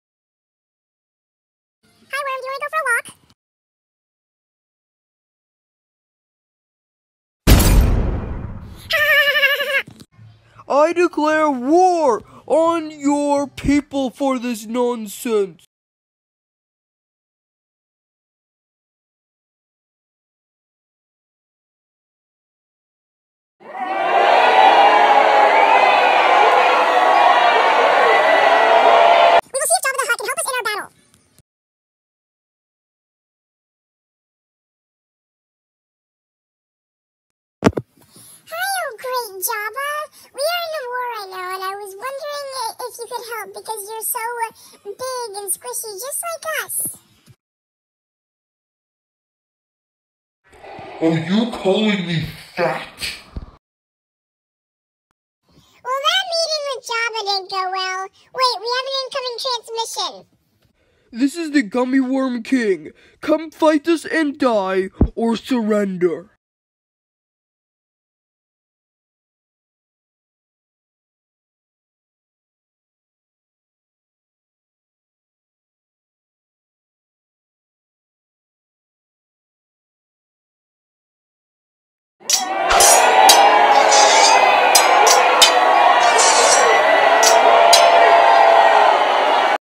Gummy War
When two types of gummies fight in gummy world, war happens. Who will win? Tell me if you want a part 2.